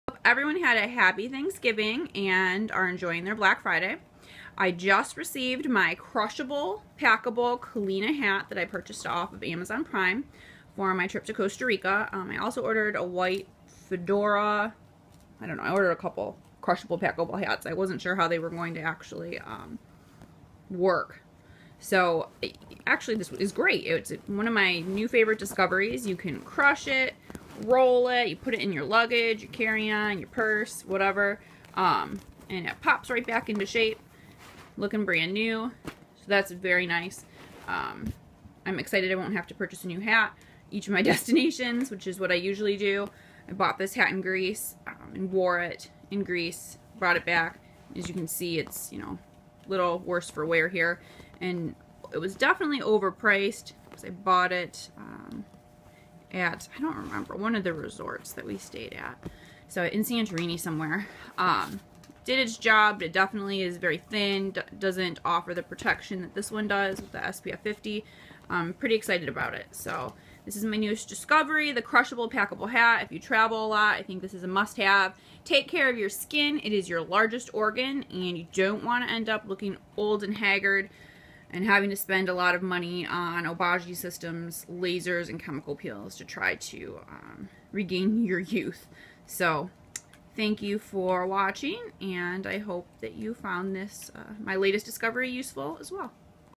Holiday Tropical Travel Essential: Crushable, Packable Panama Sun Hat Review and Demonstration
Add a crushable, packable sun hat offering SPF 50 to your Tropical Vacation packing list. These hats retain their shape during travel, which can be up to two days, come in a variety of styles and colors and can take you from the beach to a romantic dinner. I ordered a large black one as well, and am excited to channel Sydney Fashion Blogger, Antoinette Marie's, glamorous summer style while in Costa Rica. Great way to pack light.